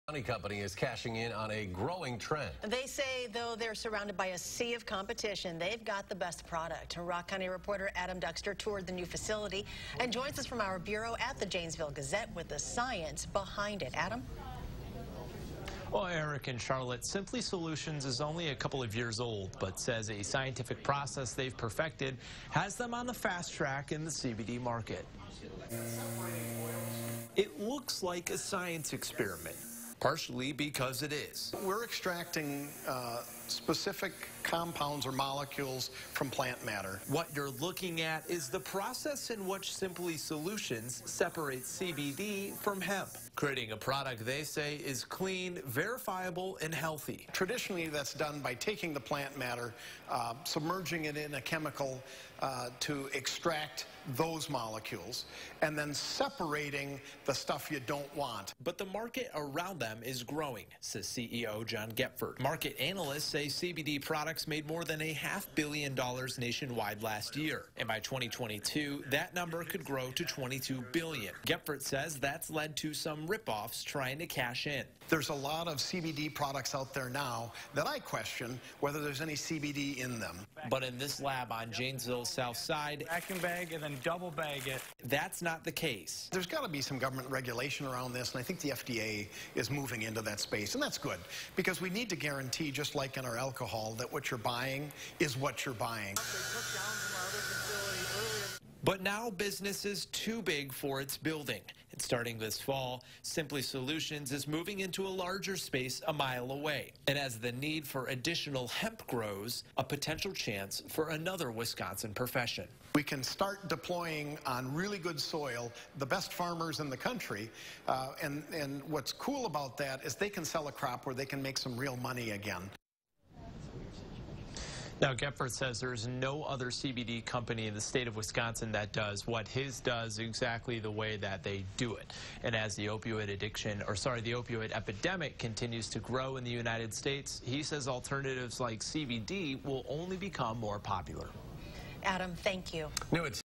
Business specializing in CBD to expand
Simply Solutions is only a couple of years old, but say a scientific process they've perfected has them on the fast track in the CBD market.